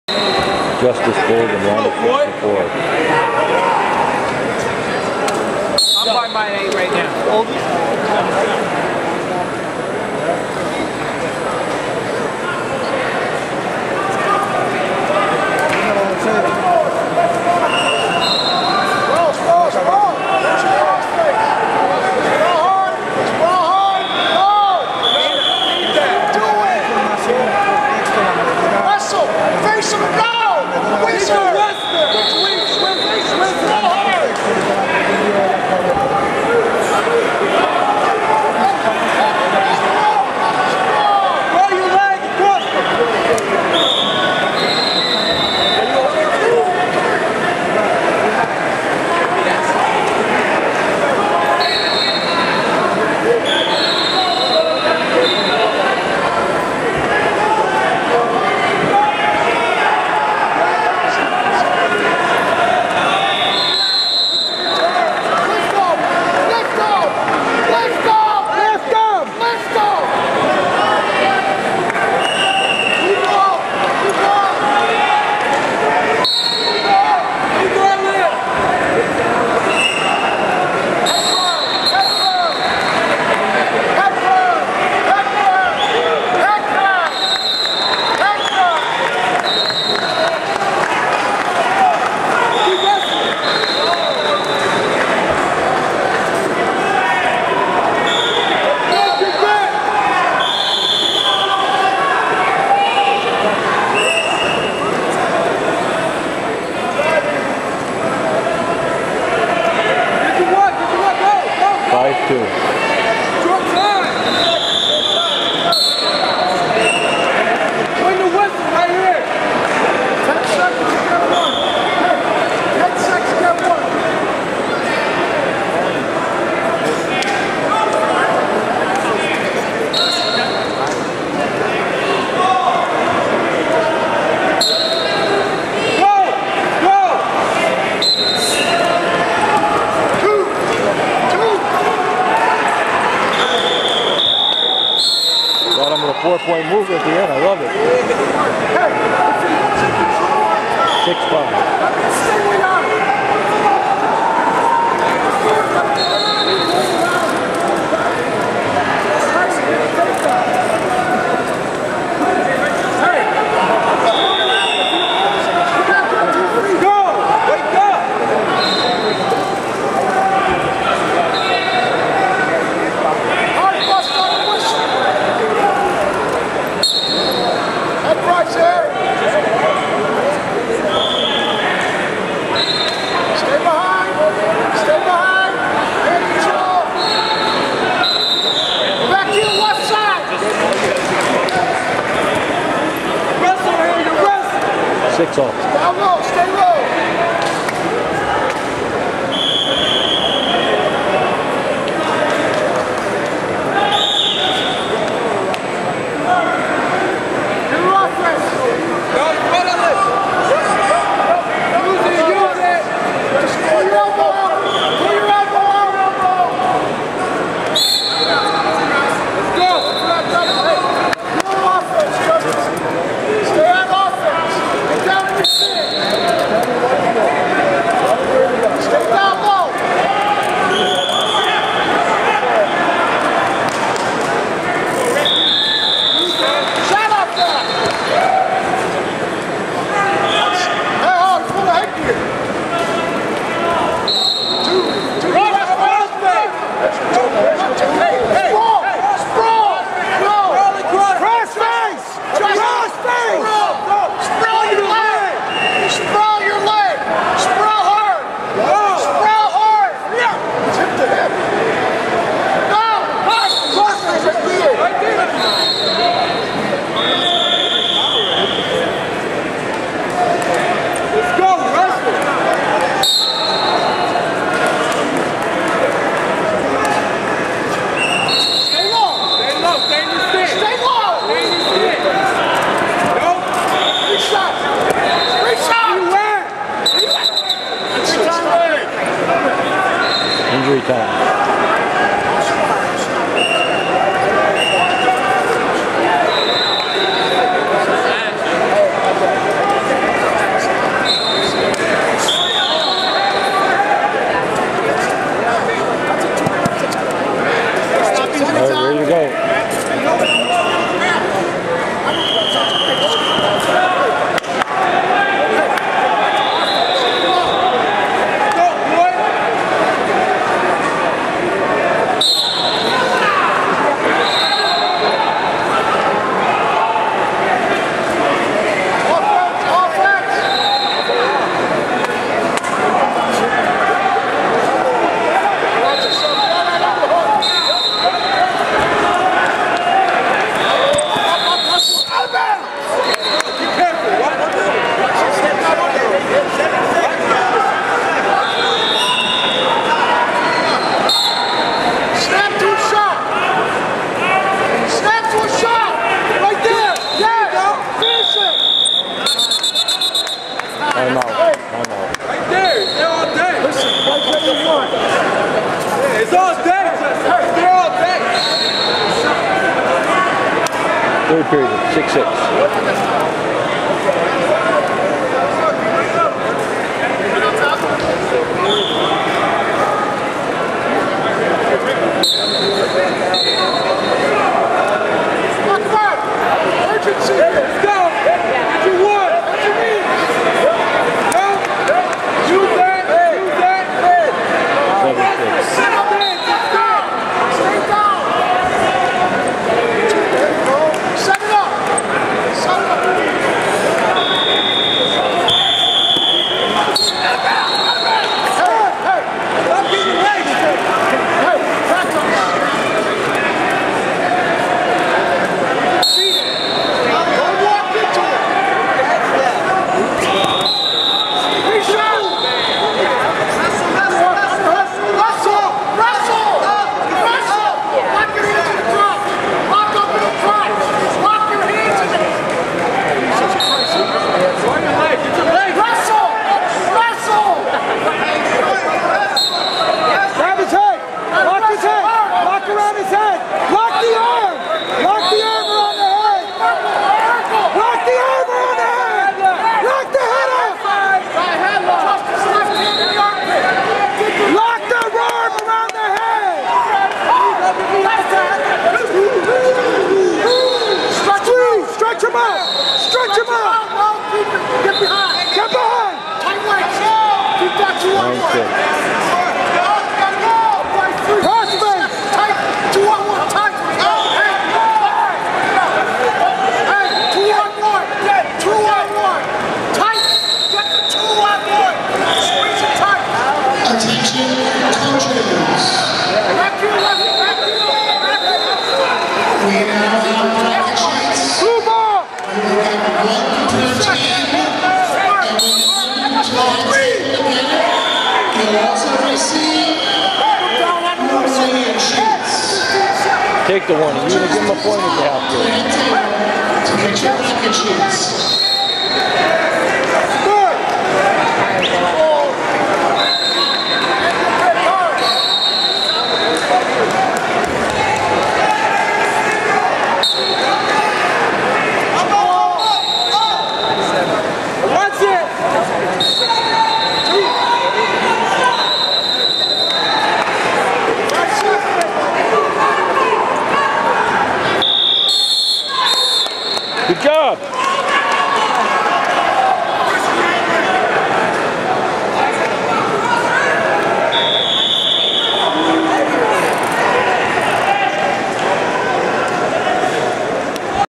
Jastice Bolden (Central) vs Alexander Palmer (MLK)
Jastice Bolden (Central) vs Alexander Palmer (MLK) in a 170 lb round of 32 match at the Eastern States Classic at SUNY Sullivan on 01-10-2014. Bolden wins 9-6.